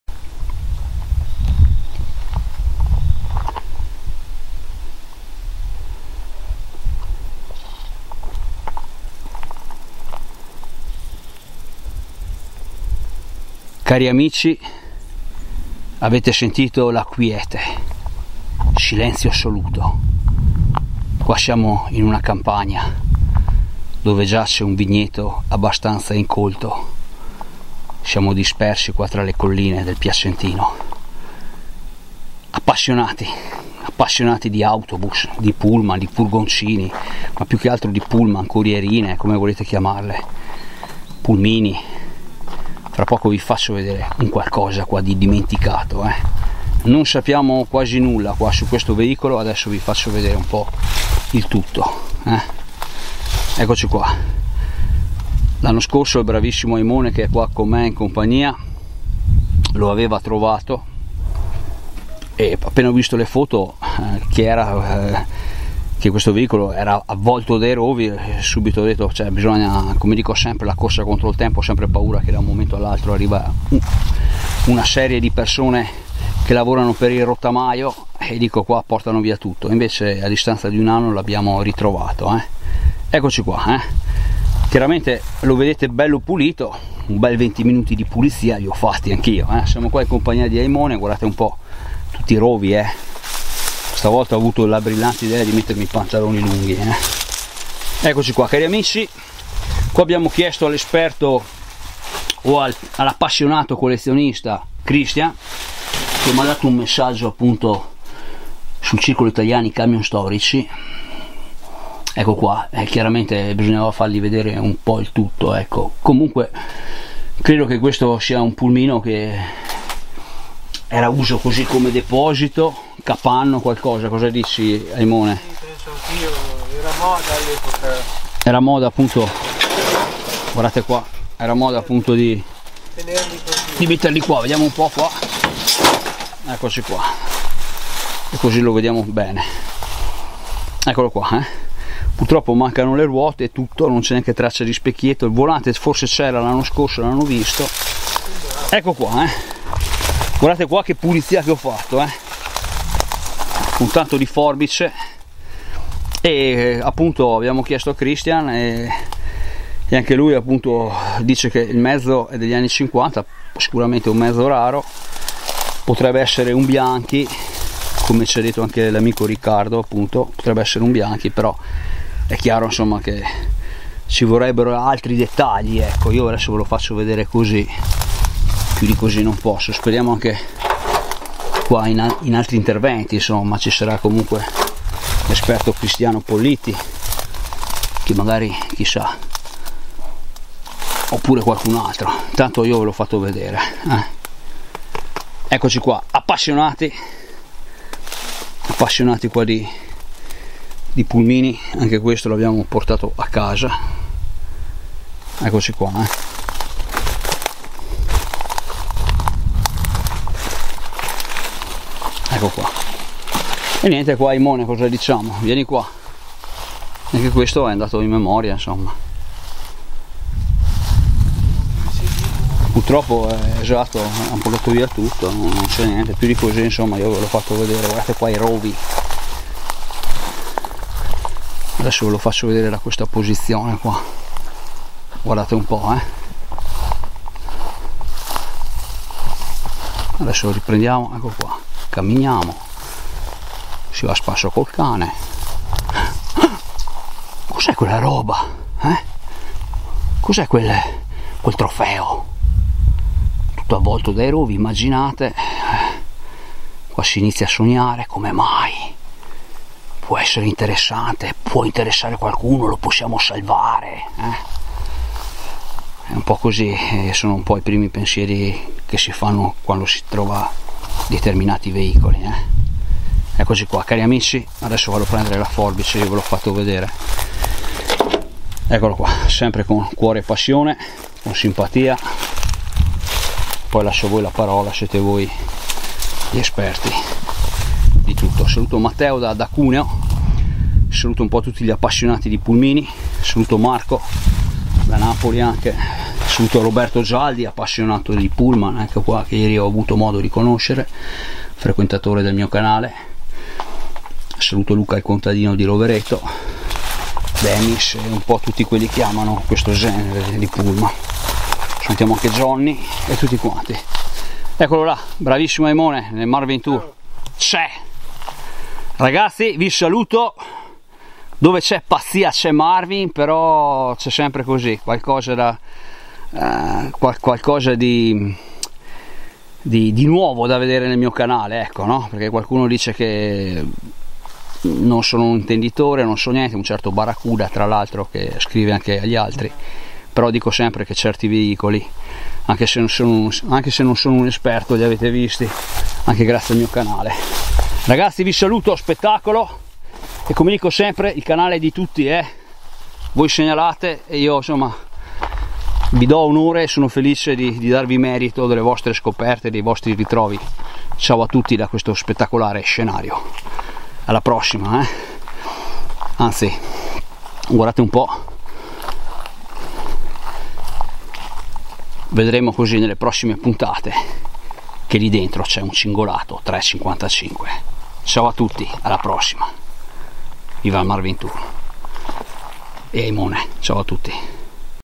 Nei rovi , nel degrado e abbandono , traccia di pullmino anni 50
18 settembre 2021  Colli piacentini

Trovato nel 2020 da AIMONE  ... Marvin lo vuole mettere in archivio ... si tratta di un veicolo abbandonato nel degrado in una traccia di vigneto abbandonato nelle colline piacentine . Si tratta di un pullmino usato come capanno o lugo di ritrovo , probabilmente anni cinquanta . Qualche amico esperto ci ha detto che potrebbe essere un BIANCHI . Difficile da decifrare come veicolo in mancanza di dettagli significativi ma lascio agli esperti la parola . Come dico sempre io sono un amante delle geometrie dei veicoli  di una volta per questo ho fatto delle ricerche in questi anni partendo davvero da zero , qualcosa ora ho imparato a cercare veicoli , diciamo sono meno superficiale ma va bene anche se non sono una enciclopedia . Buona  visione a tutti e grazie ad AIMONE per la giornata che mi ha dedicato

Your friend Marvin in the company of friend Aimone ... the search for old vehicles. We are in the hills of the province of Piacenza, sighting and meeting place of an abandoned vehicle in total decay. The model of the small minibus could be BIANCHI from the 1950s. Important details are missing to know what the name of the abandoned vehicle is. Enjoy and greetings from Italy